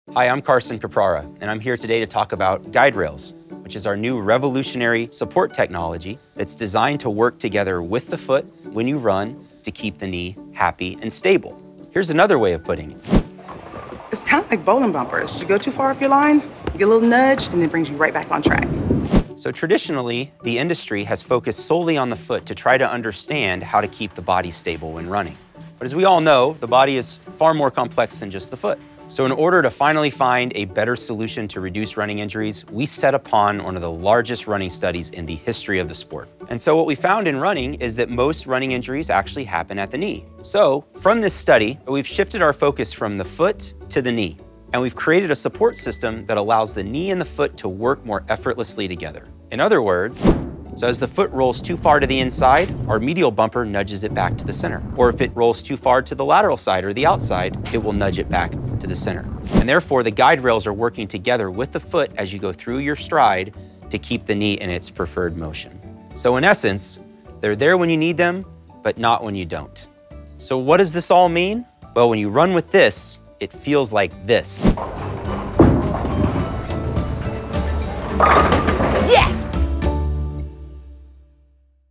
Brooks Running technology: GuideRails
Most running injuries happen at the knee. That’s why we created GuideRails® technology. GuideRails help keep your stride in a safe range of motion so you can get the most out of your run.